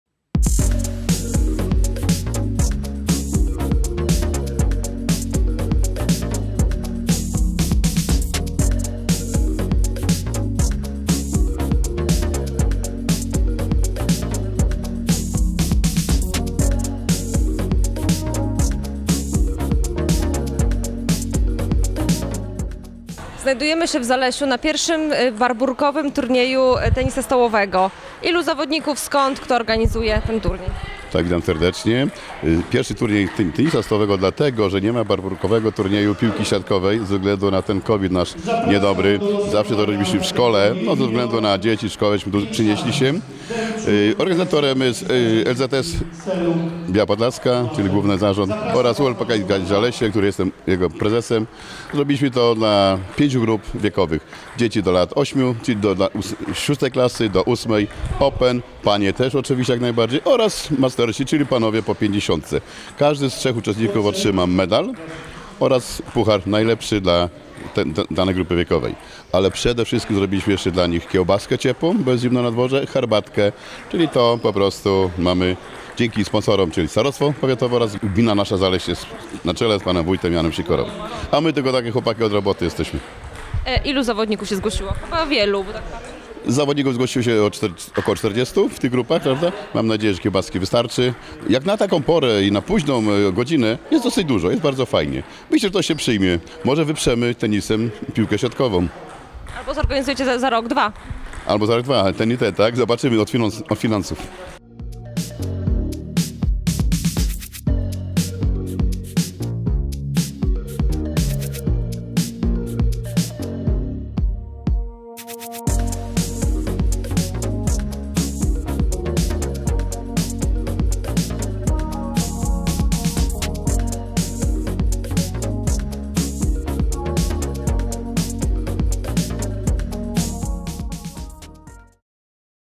Barbórkowy Turniej Tenisa Stołowego w Zalesiu
5 grudnia  w sali Gminnego Ośrodka Kultury Zalesiu rozegrany został Barbórkowy Turniej Tenisa Stołowego.